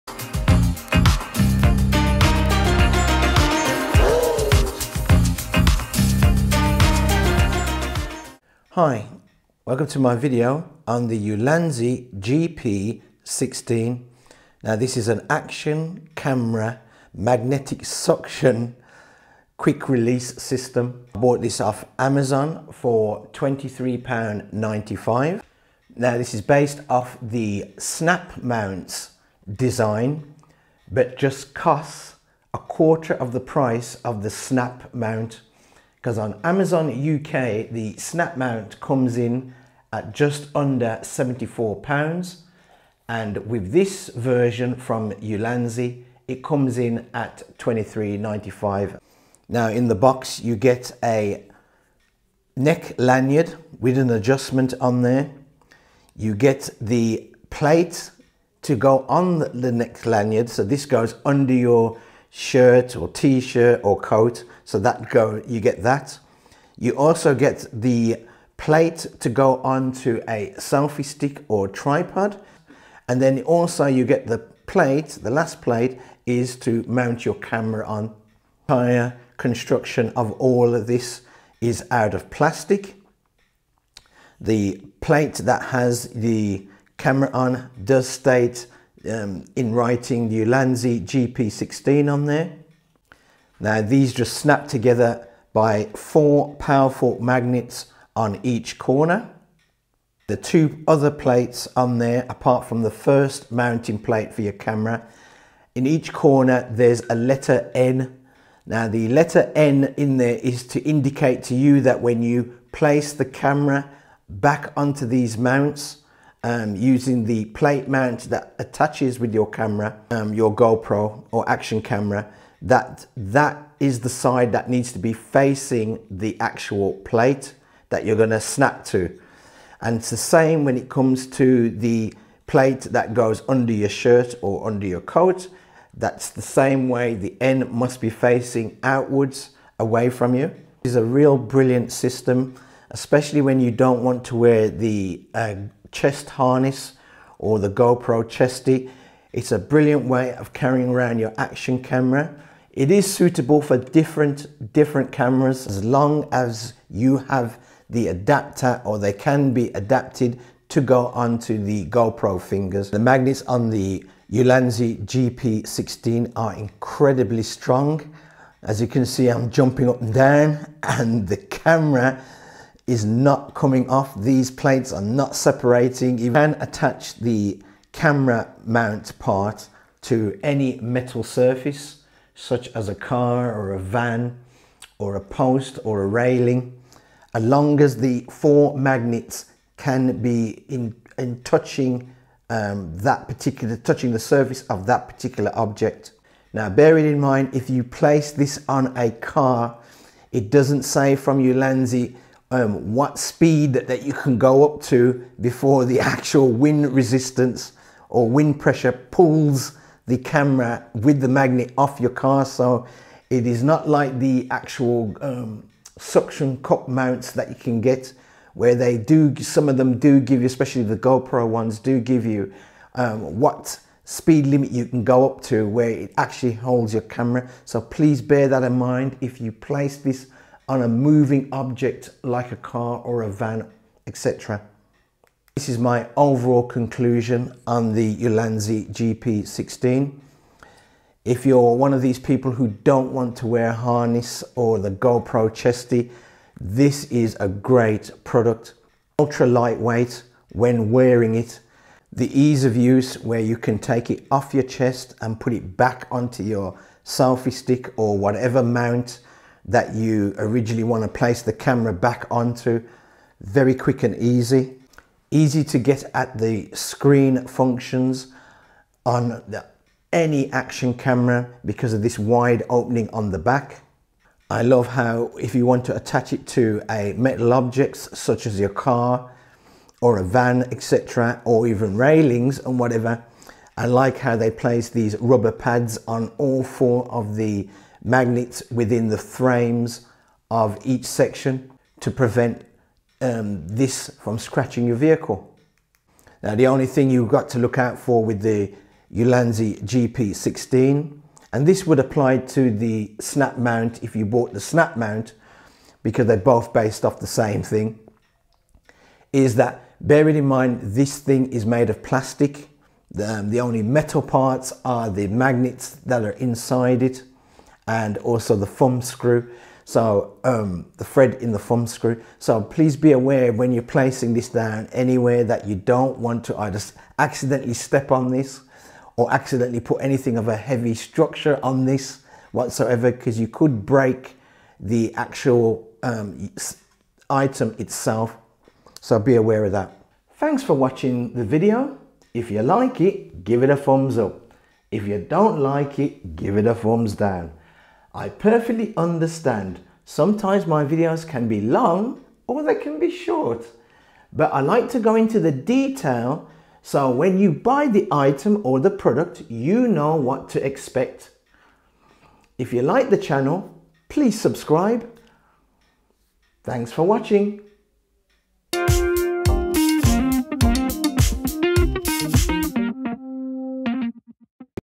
Ulanzi GP-16 (in-depth review)
Ease of Use: No need for neck braces, straps or clips, just align the 2 magnetic plates with a single tap, and you can easily shoot from the first angle!
⚡Super Time-Saving: No tools are required, and it can be quickly released by using strong magnetism to achieve efficient shooting needs. Package Contents: Snap Plate*1, Adapter Back Plate*1, Back Plate*1, Lanyard*1.
⚡Multiple Ways to Use: 1. Directly attach to clothes for first-person shooting. 2. Install it on the selfie stick or tripod of the gopro interface to get a wider viewing angle. 3. Shoot directly on metal surfaces, such as iron mesh, railings, and cars.
⚡Strong Magnetic Force: High-strength magnet, the static adsorption load can reach 20N, and the normal exercise load can be 250g. Please note: Not suitable for extreme sports such as parkour, motorcycles, cycling, etc.
⚡Widely Compatible: The magnetic stand is compatible with all GoPro Hero 10 9 8, DJI Osmo Action, Insta 360 One R/RS, Go 2.